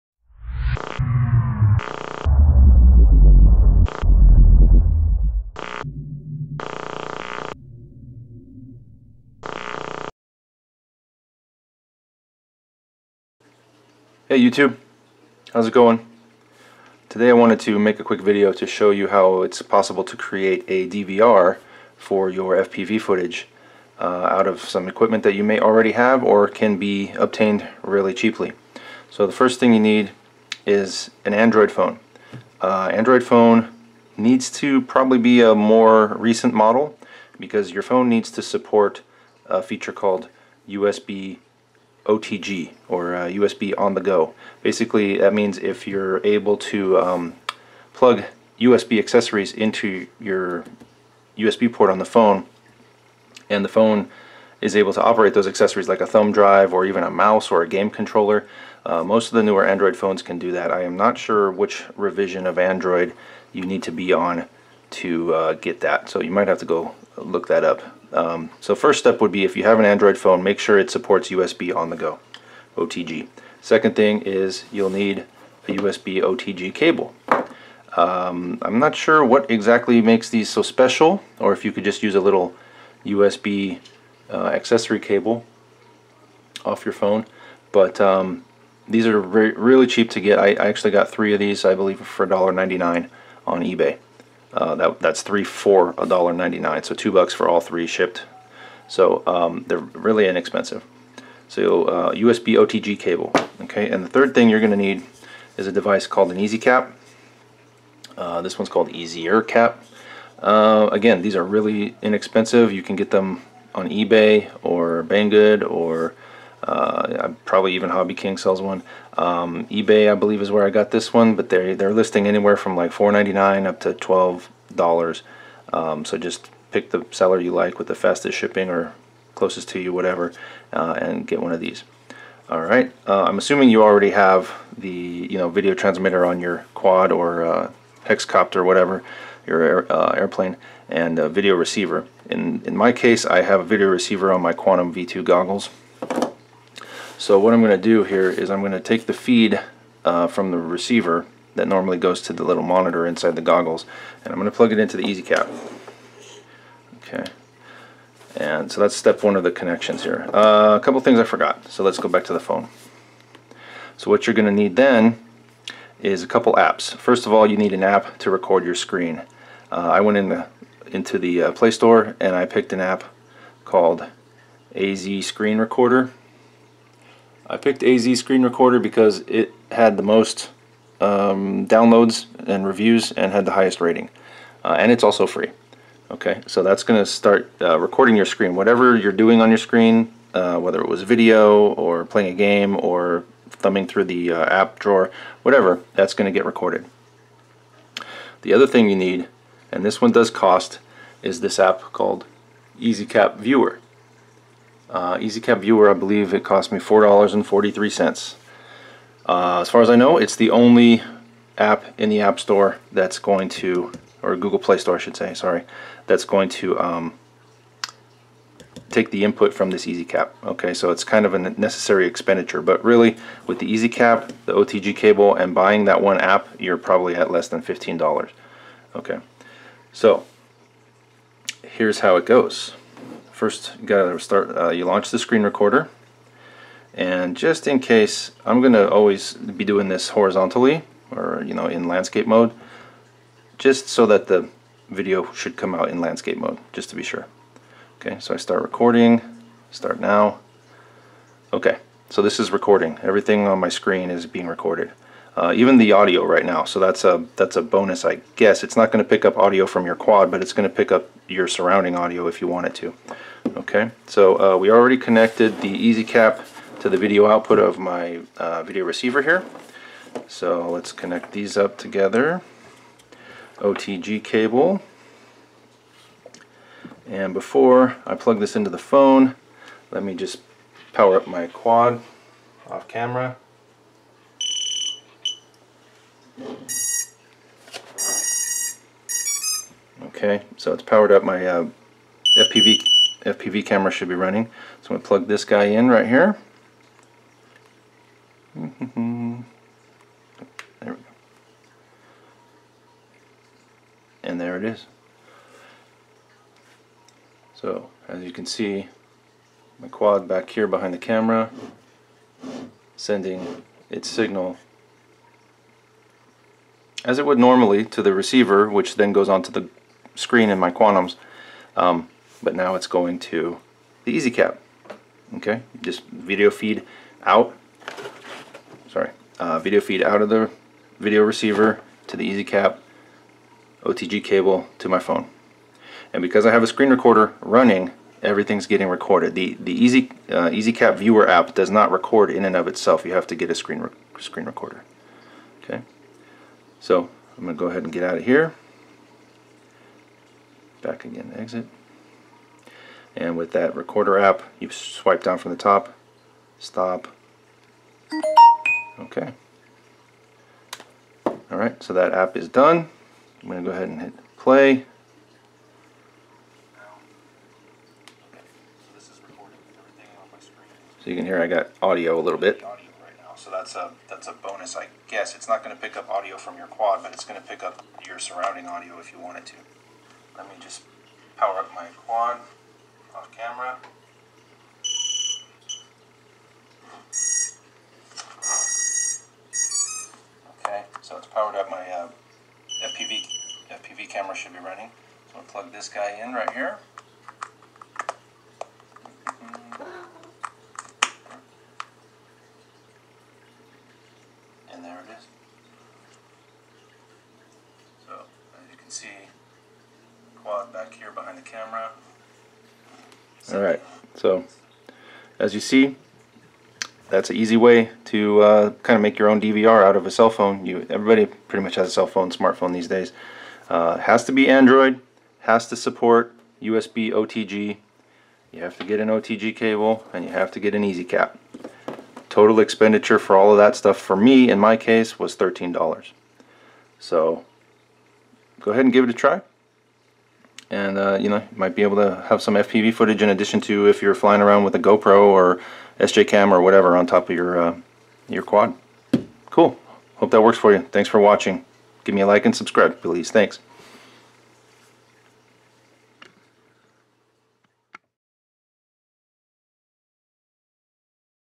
Cheap and Easy FPV DVR using your Smartphone
A short how-to on making a DVR for your FPV experiences using an Android Smartphone, an OTG cable, an EasyCap USB Transcoder, and 2 apps from the Play Store.  Total cost is about $15 dollars!

OTG Cable example

EasyCap example